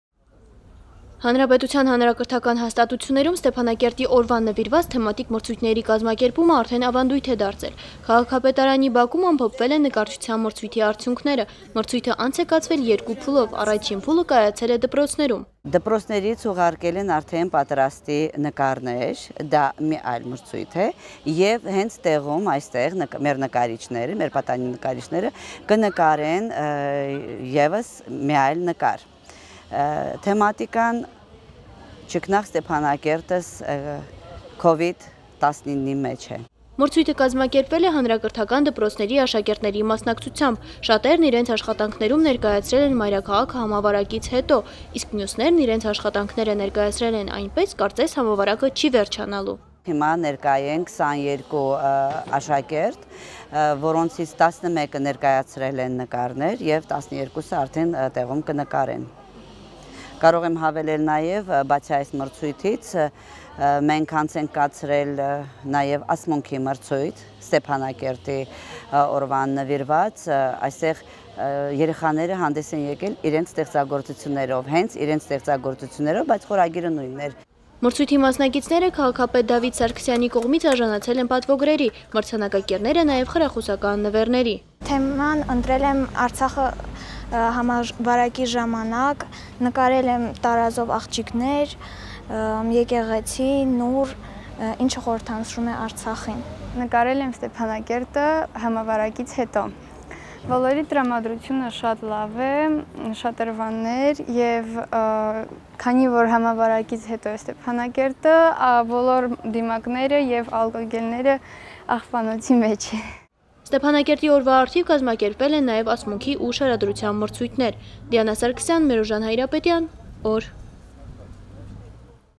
Ստեփանակերտցի երեխաներն են պատկերել մայրաքաղաքը՝ իրենց աչքերով
Քաղաքի օրվան ընդառաջ՝ քաղաքապետարանի տարբեր բաժիններ տարբեր թեմաներով ստեղծագործական մրցույթներ են հայտարարել: Դրանց մի մասն ամփոփվեց այսօր: Ստեփանակերտցի երեխաներն են պատկերել մայրաքաղաքը՝ իրենց աչքերով: